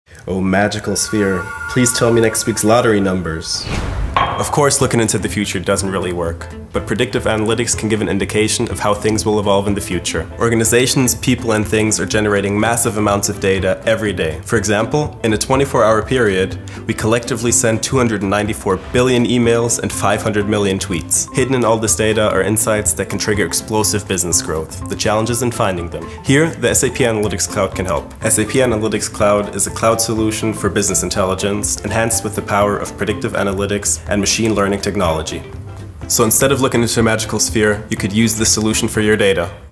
What is SAP Analytics Cloud☁️ | Tech Explained
Have you ever wished you could see into the future? 🔮
Organizations, people, and things generate huge amounts of data. SAP Analytics Cloud can analyze these and support you in predicting the future. It is not a magical sphere, but still pretty fascinating.

📸: Justin Rodney